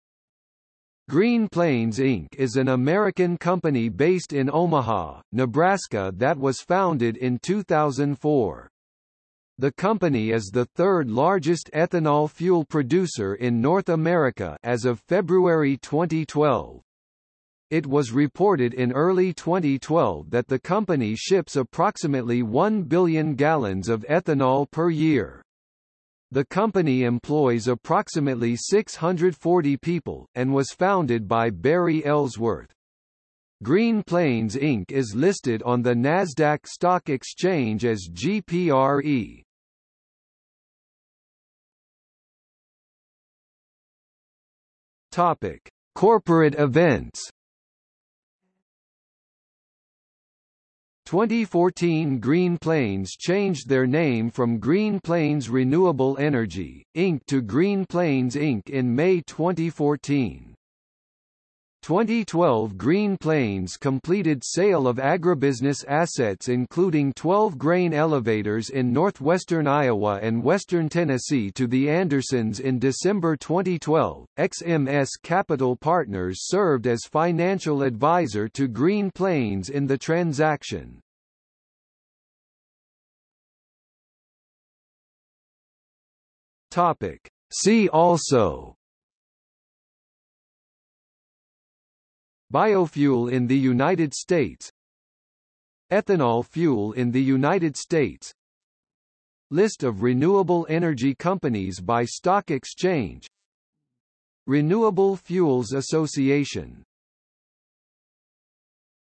Green Plains Renewable Energy | Wikipedia audio article
00:00:41 1 Corporate events
00:01:23 2 See also

- Socrates

SUMMARY
Green Plains Inc. is an American company based in Omaha, Nebraska that was founded in 2004. The company is the third largest ethanol fuel producer in North America (as of February 2012). It was reported in early 2012 that the company ships approximately one billion gallons of ethanol per year. The company employs approximately 640 people, and was founded by Barry Ellsworth. Green Plains Inc. is listed on the NASDAQ Stock Exchange as GPRE.